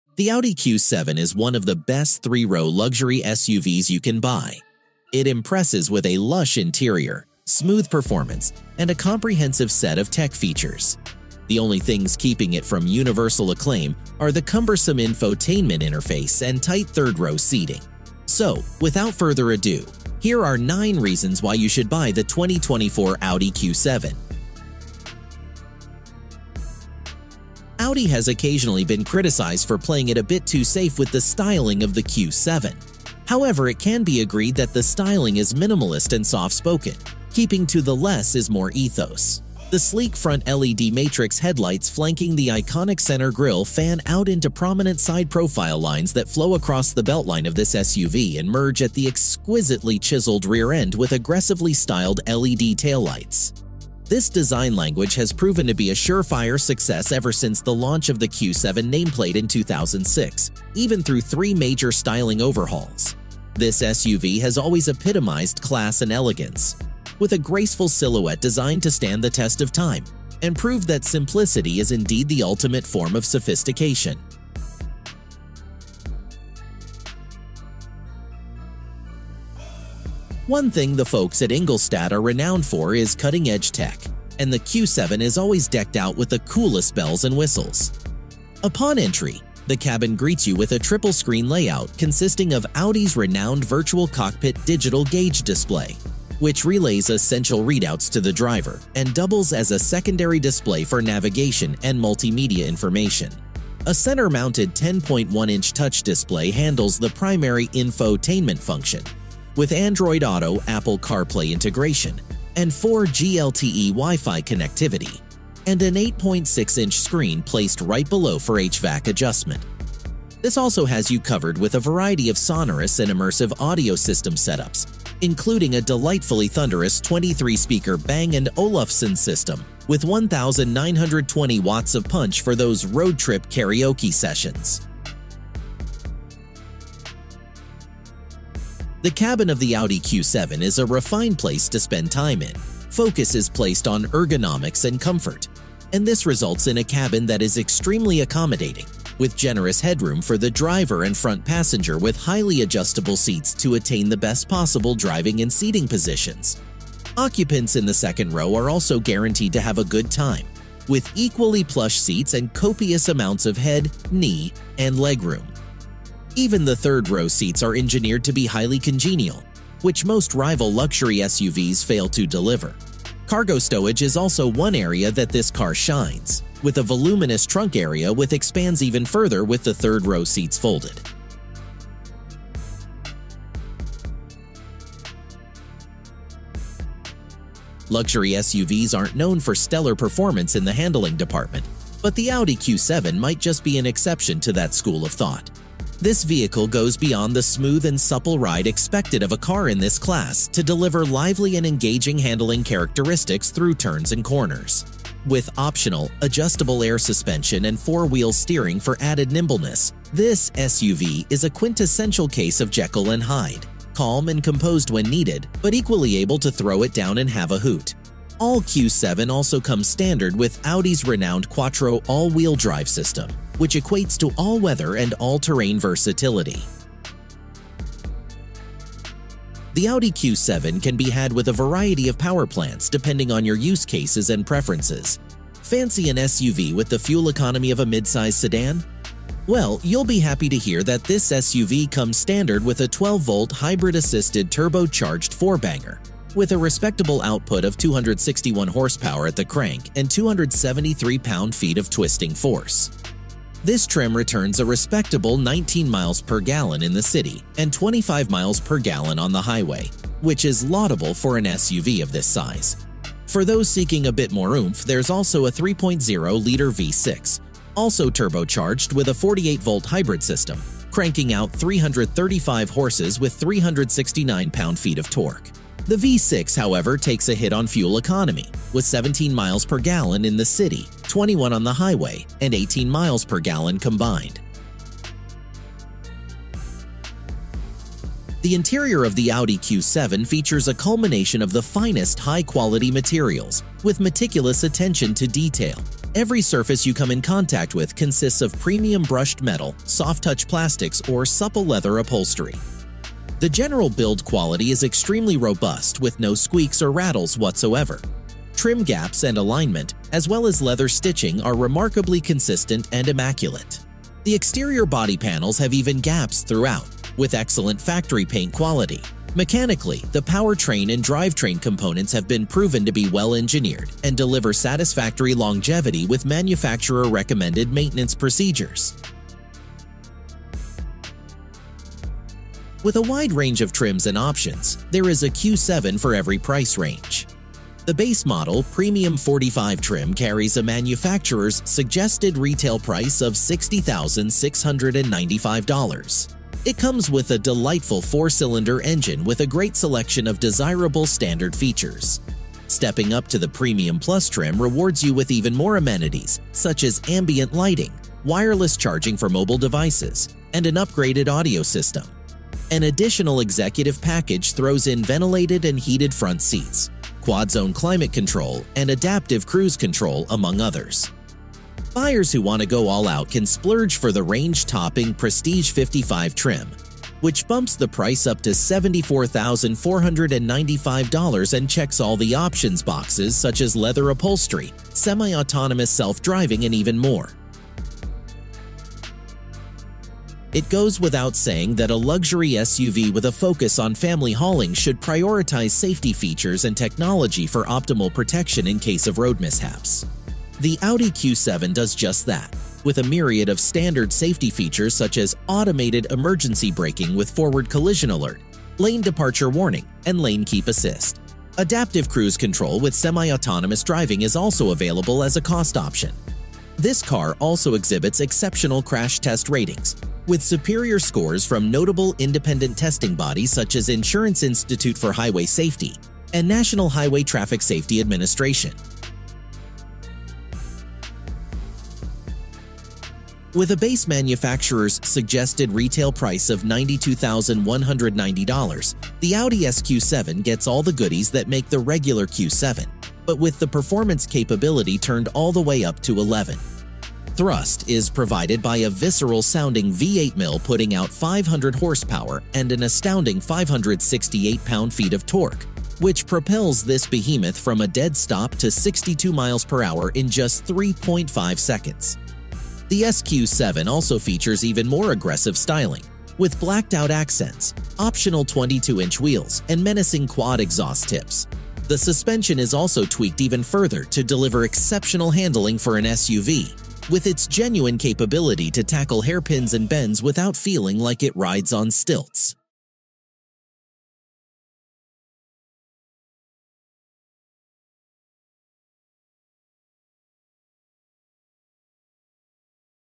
9 Reasons Why You Should Buy The 2024 Audi Q7
The Audi Q7 is one of the best three-row luxury SUVs you can buy. It impresses with a lush interior, smooth performance, and a comprehensive set of tech features. The only things keeping it from universal acclaim are the cumbersome infotainment interface and tight third-row seating.
So, without further ado, Here Are 9 Reasons Why You Should Buy The 2024 Audi Q7.